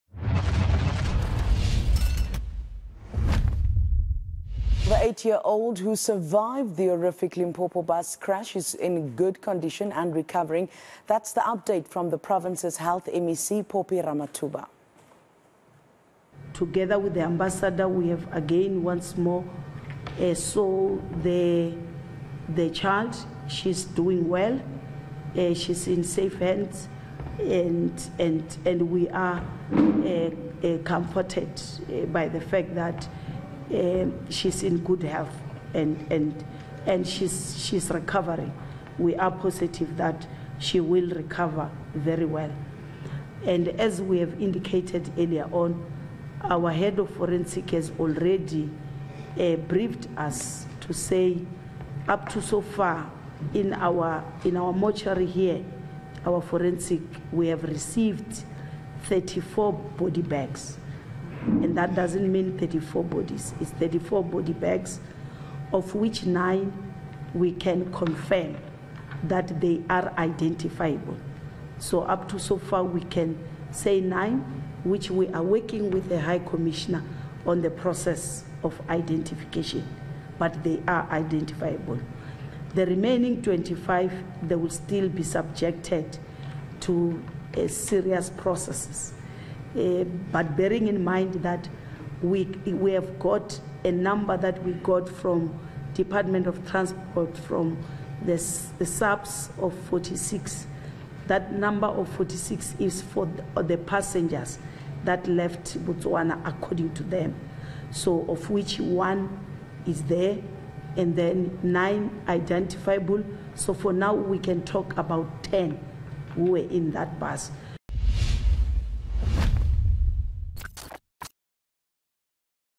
8 year old in Mokopane bus crash recovering
The 8-year-old who survived the horrific Limpopo bus crash is in good condition and recovering. That is according to Health MEC, Phophi Ramathuba. She says the country is working with the Botswana government to identify those who died.